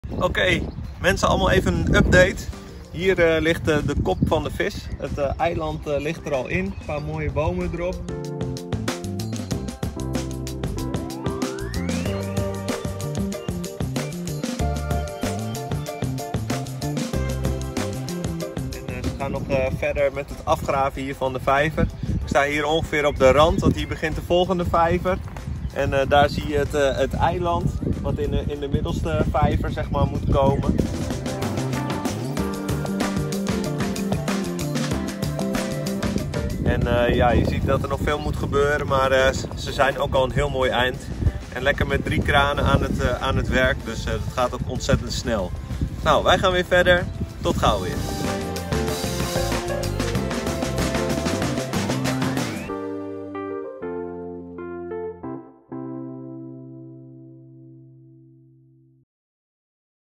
#2 Uitbreiding Vispark: Grote vijvers, grote vissen
Een korte update over de grote uitbreiding van vispark Toms Creek.

Er worden 4 grote nieuwe vijvers gerealiseerd op een terrein van 40.000 m2.

De vier grote vijvers hebben allen een nieuw visconcept:

- een karpervijver voor de recreant met grote karpers,
- een 'big game' vijver met grotere steuren, karpers en meervallen, groter dan de huidige avonturenvijvers,
- een meervalvijver met diverse soorten meervallen
en als laatste de 'monster creek' met de grootste vissen tot boven de 2 meter!

Realisatie zomer 2021.